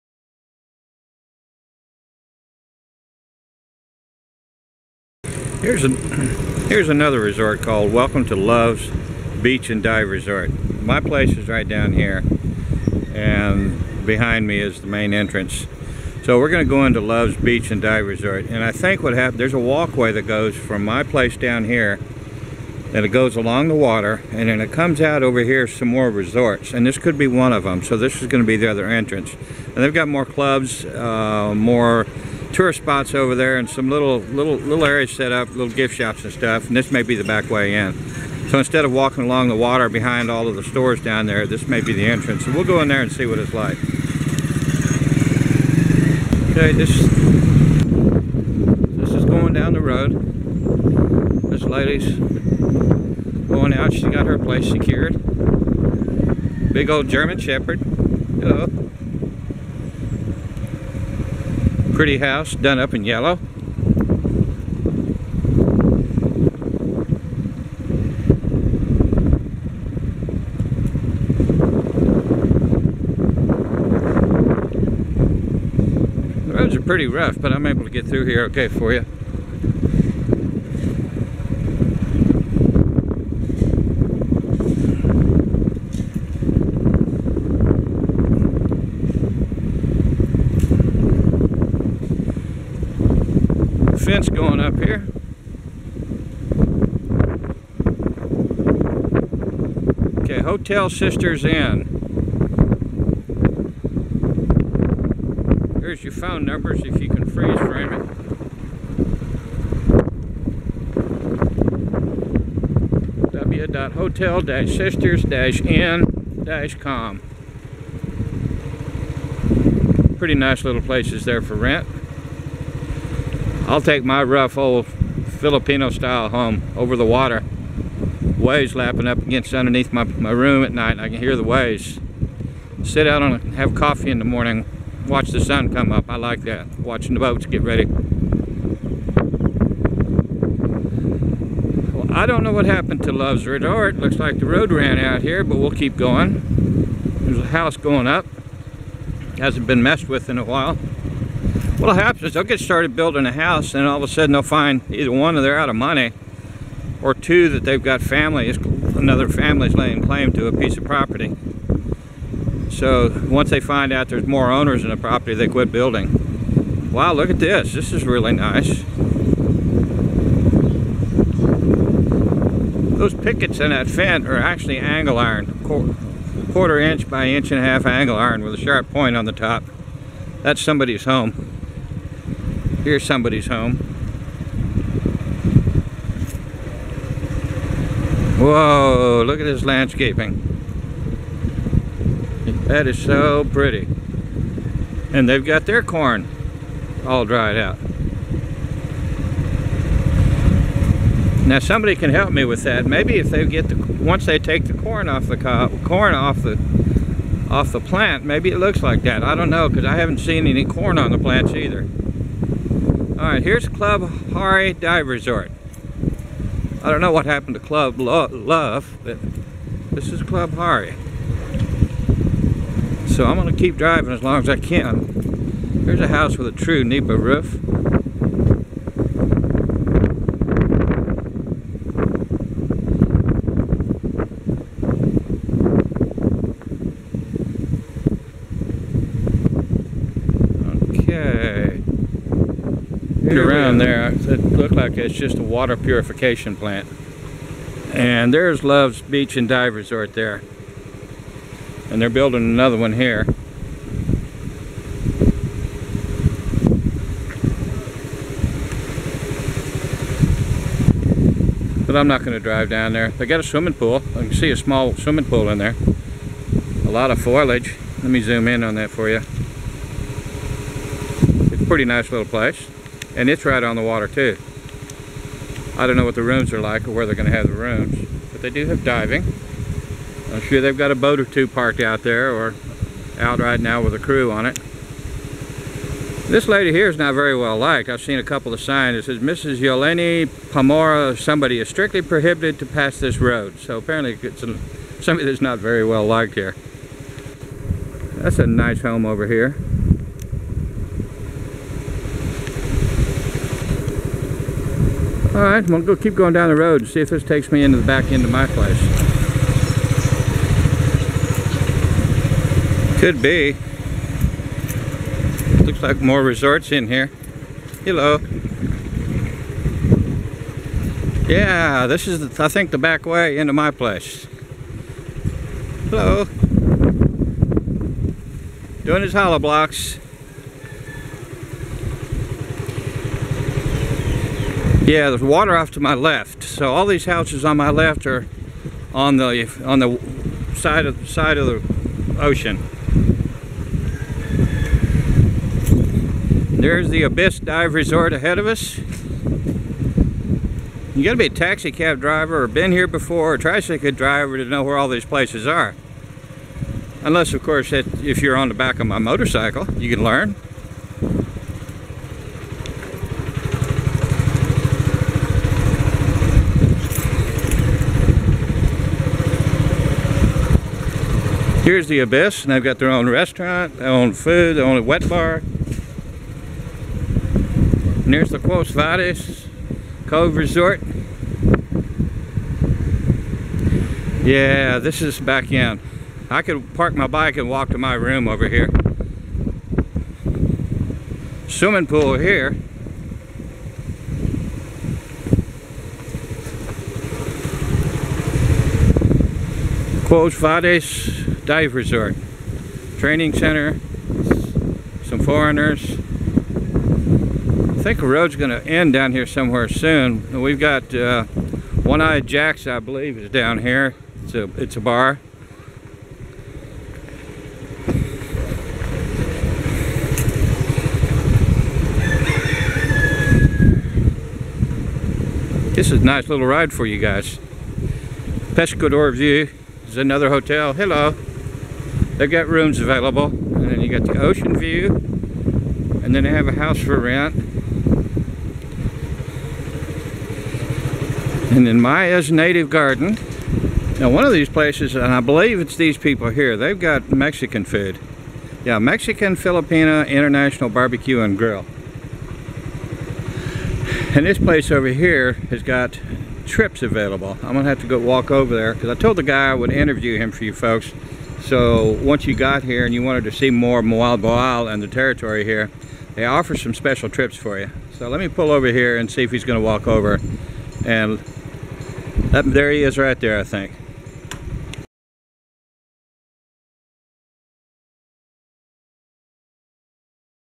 Moalboal Panagsama Beach, trekking the other side ~ Philippines tourism ~ My Motorcycle Adventures
Philippines tourism ~ Today I found a gravel road that leads to the other side of Panagsama Beach from where I'm staying here at Moalboal Cebu.  In my previous video, there are some concrete stairs on the ocean front that lead up to these dive resorts, hotels, resorts, restaurants, and night clubs. ~ My Motorcycle Adventures